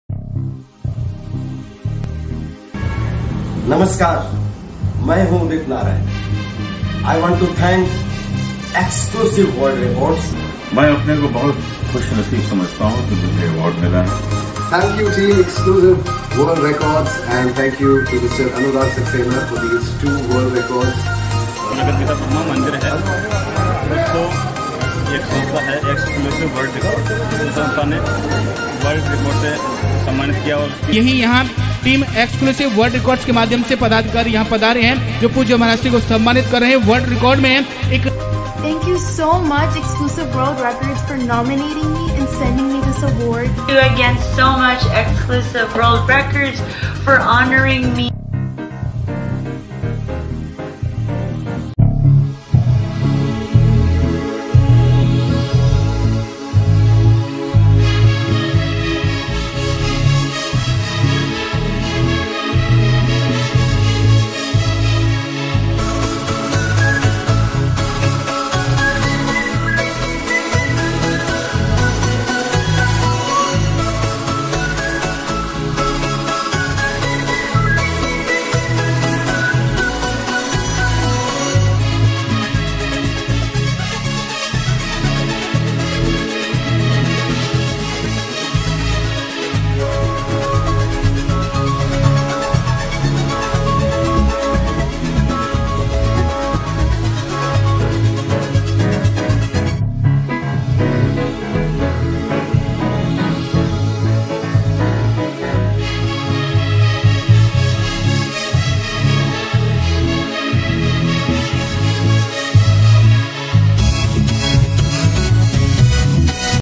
"Beauty Beyond Borders: The Multifaceted Journey of Natalia De Graaf Smolska"
Natalia De Graaf Smolska (Natasha) of Den Haag, Holland, Netherland has been honored by Exclusive World Records for being a multi-talented woman. As a Doctor of Aesthetical Medicine and owner of Liquid Beauty Clinic, She combines medical expertise with a passion for beauty. Natalia is not only a skilled practitioner but also a versatile artist, showcasing her talents as a singer, photo model, and achieving the prestigious title of 1st Runner Up at Miss Classic Grand Sea World in 2022."